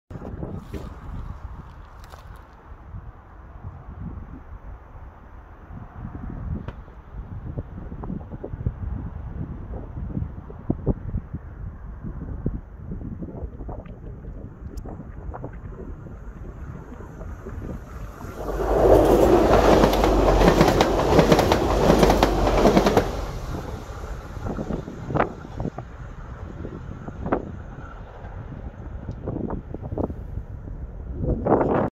AGC 82765 SNCF TER HAUTS DE FRANCE près de THIERNU le 0 février 2021
Sur la ligne  LAON  HIRSON